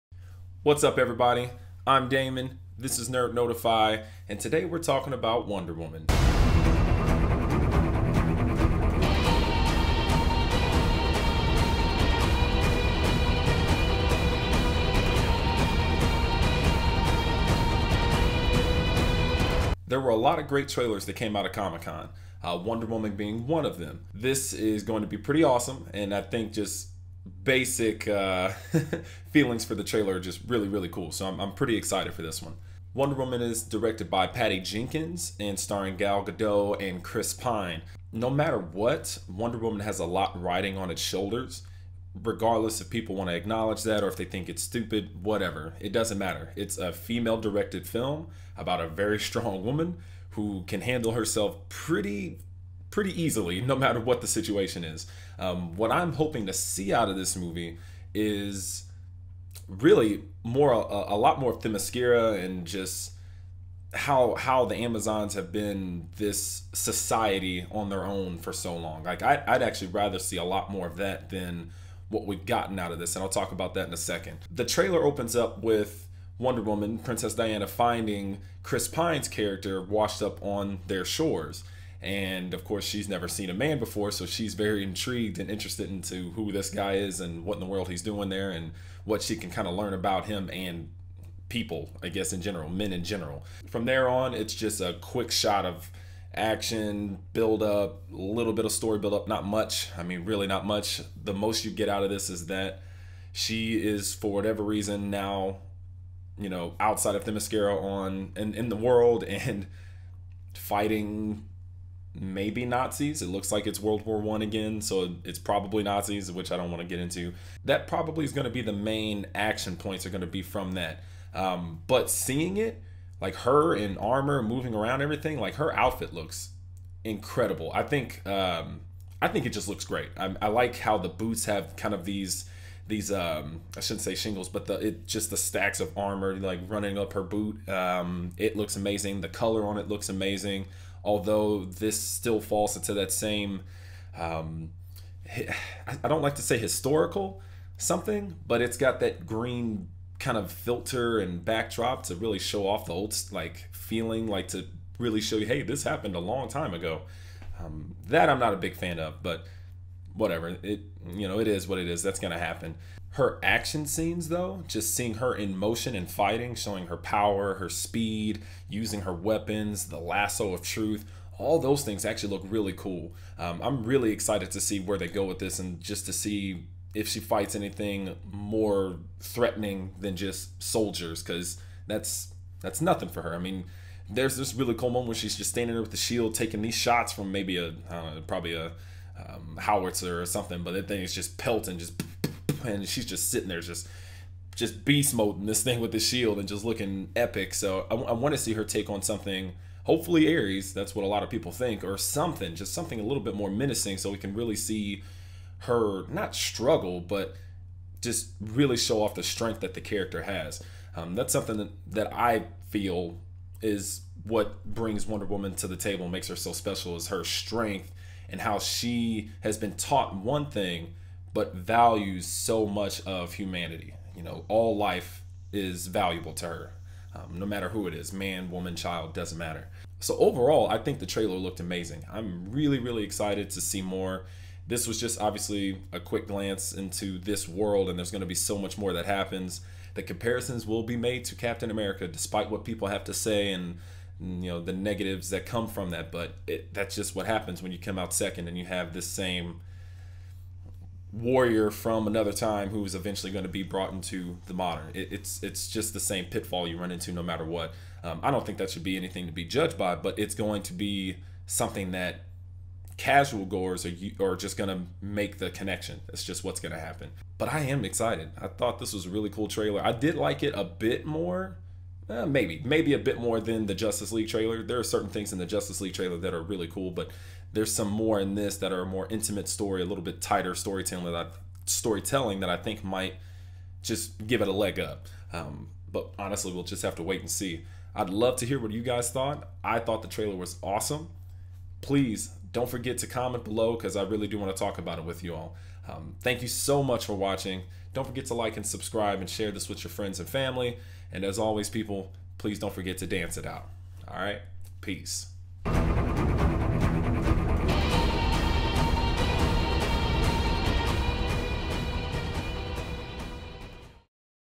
Wonder Woman Trailer Review
Comicon has come and gone for another year but this Wonder Woman trailer may last an eternity.

Note: I said Wonder Woman may be fighting Nazis, but if this is WW1 than they would be just German Soldiers. My bad!

Follow me on all those social networks you kids love.